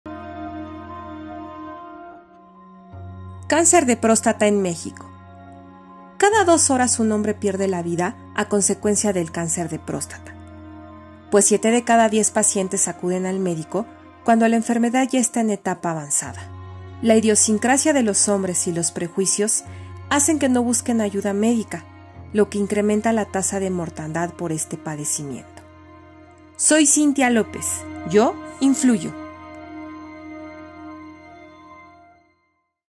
Cáncer de próstata en México
El cáncer de próstata representa el primer tumor en el hombre por arriba de los 50 años y equivale al cáncer de mama.